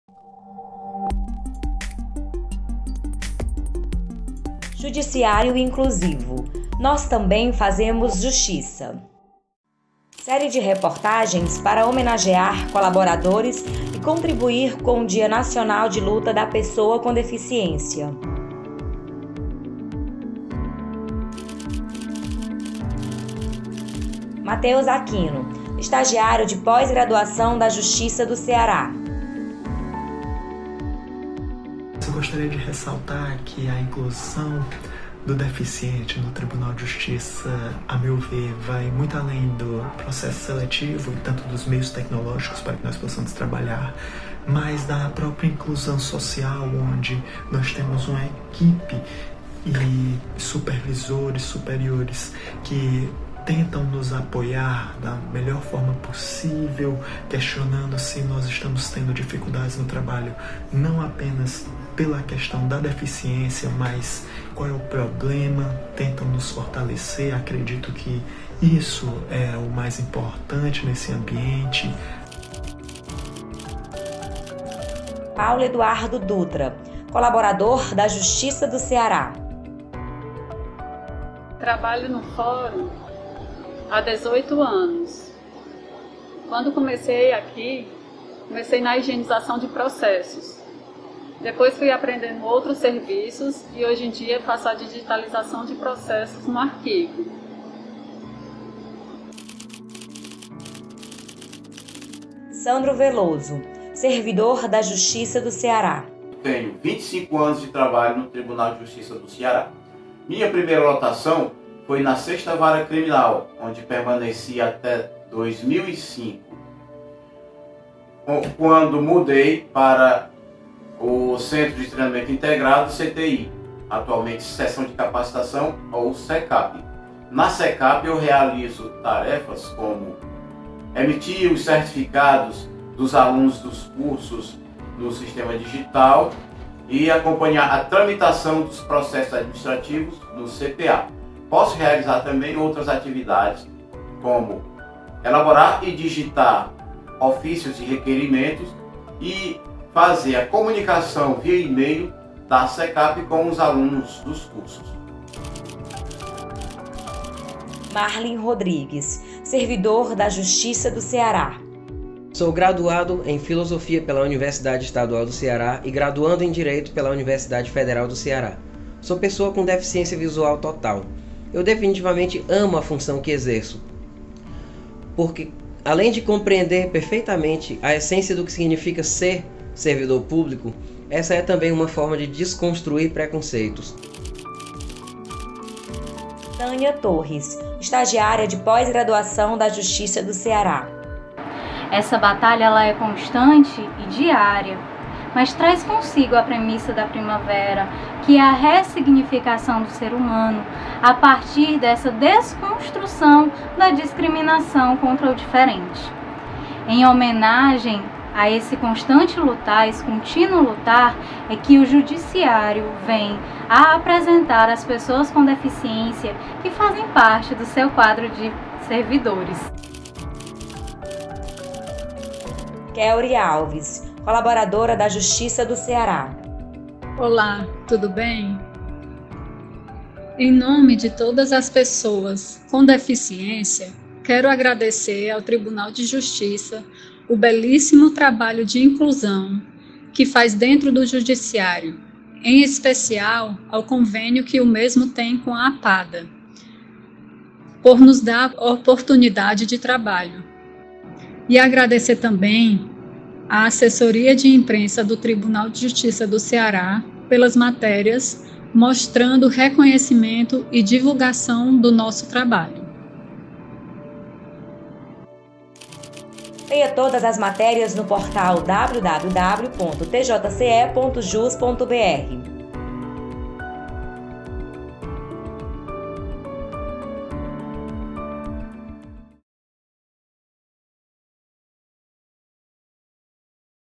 Série “Judiciário Inclusivo” destaca trabalho realizado por pessoas com deficiência no TJCE
Para finalizar a série de reportagens “Judiciário Inclusivo: Nós também fazemos Justiça”, publicamos vídeo institucional com testemunhos de vários colaboradores sobre o trabalho que desenvolvem. Eles aproveitaram a oportunidade para agradecer a iniciativa do TJCE.